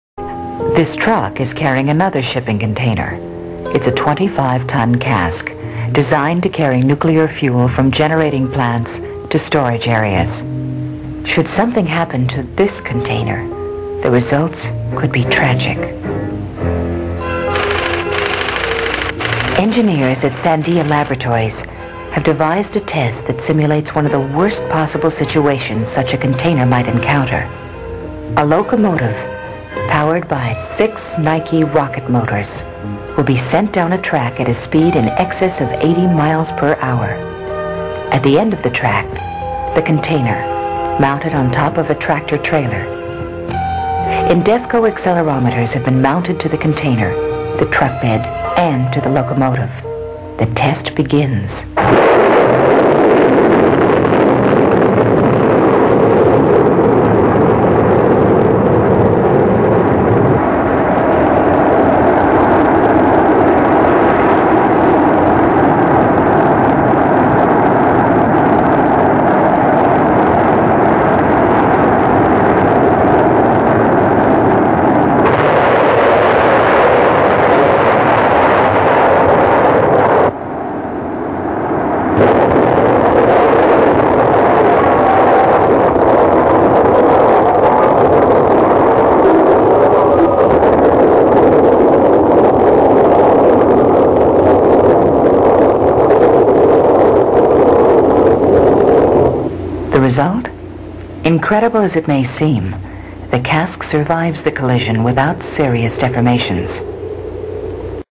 Restored Version 720HD - Rocket Powered Train Impact Test of Spent Nuclear Fuel Shipping Cask
VIDEO ENHANCED AND RESTORED FROM ORIGINAL

A marketing video made from a Sandia National Laboratories 1978 archived case integrity test of a container designed to transport nuclear waste. Obviously, this was the product of engineers with too much time on their hands and few extra Nike rocket motors laying around cluttering up their space. Add a few too many beers on a Friday night planning session and you now have all the ingredients for this test being conceived and conducted under the auspice of real science. Of course, they could have just dropped the container from a crane or maybe even just tried the train approach using the diesel motors they typically come with but where's the fun in that?